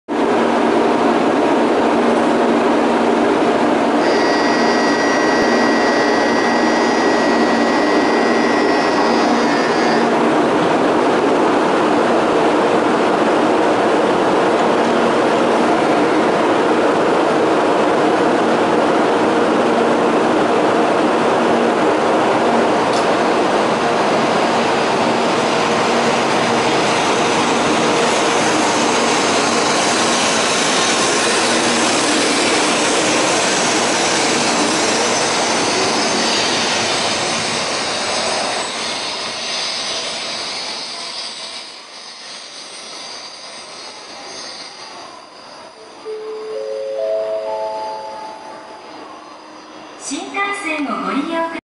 13.11.30「さくら543号さん、岡山駅を発車。」の巻。
岡山駅でさくらちゃんから降り、四国方面へと向かうわんこ・・♪

撮影カメラ：SONY DSC-WX170